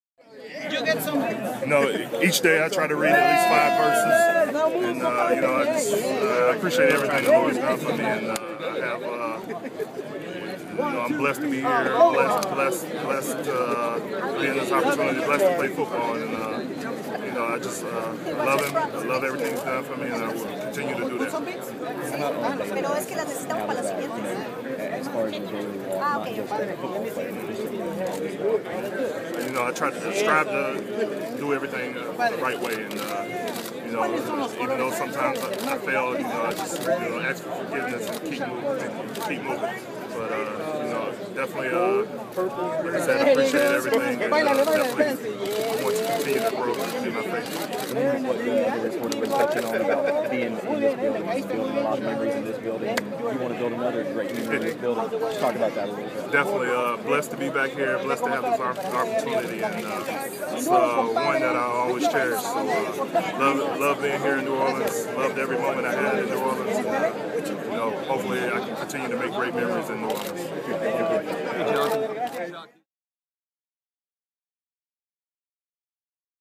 Jonathan Goodwin - Super Bowl XLVII Media Day
Jonathan Goodwin talks about reading the Bible.